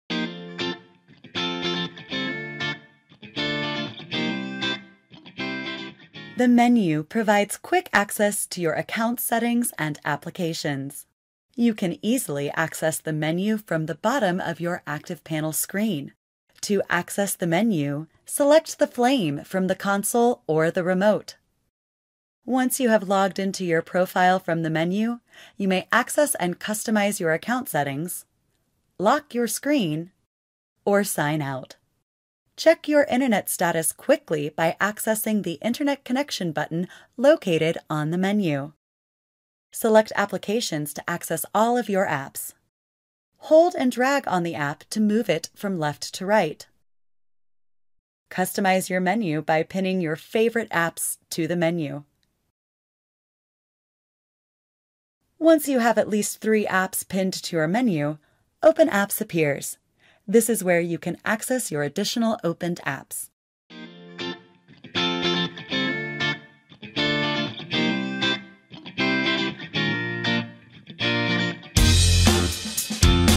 Menu
Learn how to access your applications, files, and more on the Promethean ActivPanel's menu.